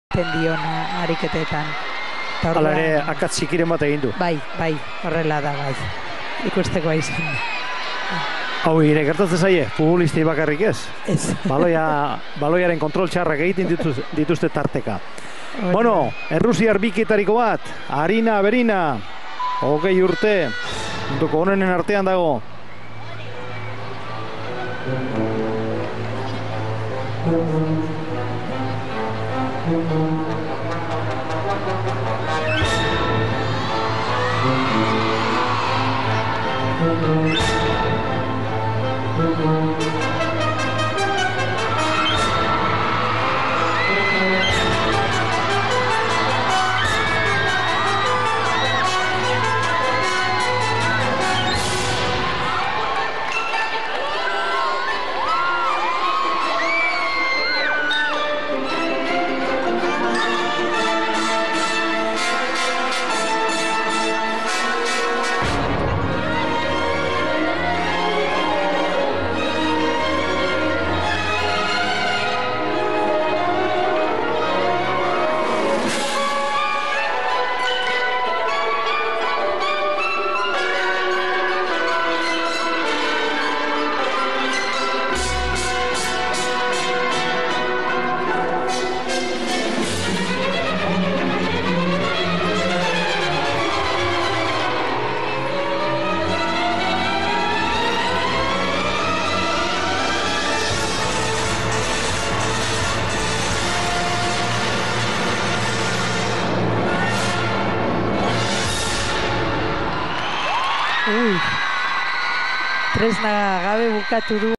Arina Averina (2018) Ribbon (Gala) | EUSKALGYM · BILBAO (SPAIN)
Cr. EiTB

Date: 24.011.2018

Score: NONE

Age: 20years.

ATTENTION